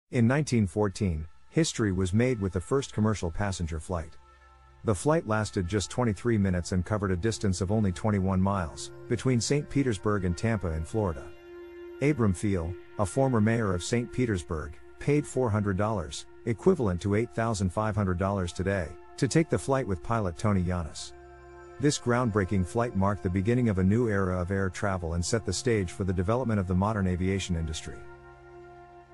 The Untold Story of the First Commercial Flight: Secrets Revealed#MysteryUnveiled
The first commercial flight took place on January 1, 1914, when a Benoist XIV biplane flew from St. Petersburg to Tampa, Florida. The flight, which was operated by the St. Petersburg-Tampa Airboat Line, was a short 23-minute journey, but it marked the beginning of commercial air travel. The flight was piloted by Tony Jannus, who was a famous aviator at the time, and it carried one passenger, former St. Petersburg mayor Abe Pheil. The flight cost $400, which was a significant amount of money at the time, but it was worth it for the novelty of flying. Today, commercial air travel is a common mode of transportation, and millions of people travel by plane every day.

"The Untold Story of the First Commercial Flight: Secrets Revealed"